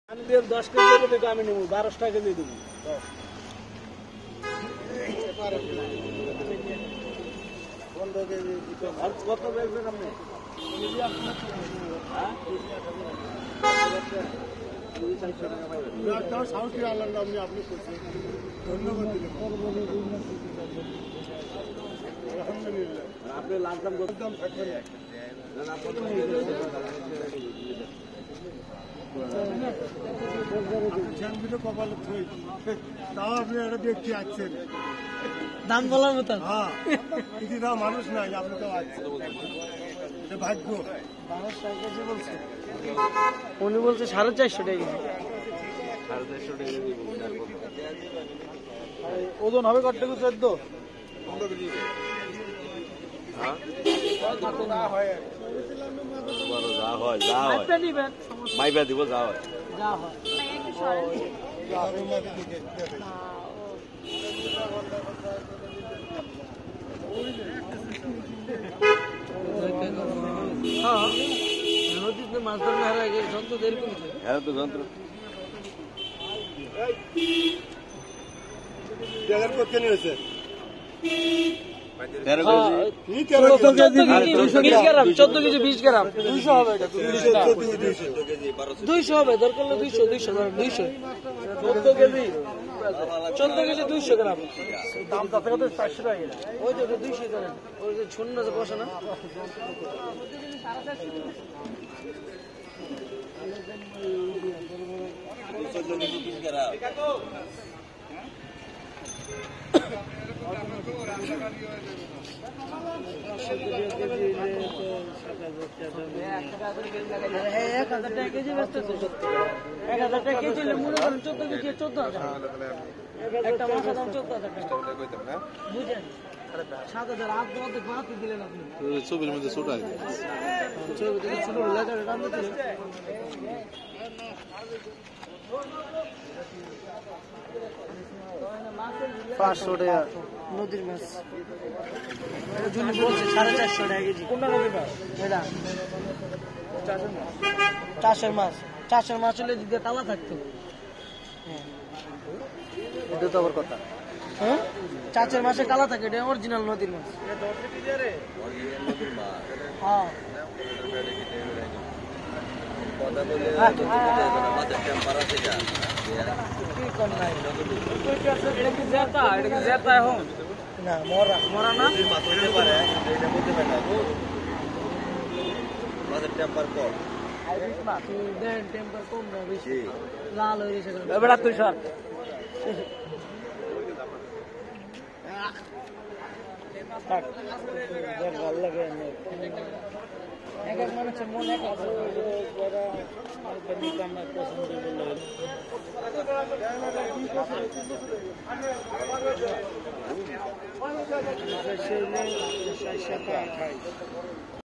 ১৪.৫ কেজি ওজন এর একটি কাতল মাছ
১৪.৫ কেজি ওজন এর একটি কাতল মাছ জনসমুদ্রের মধ্যে থেকে ৫০০ টাকা কেজিদরে ৭০০০ টাকায় বিক্রি করা হলো।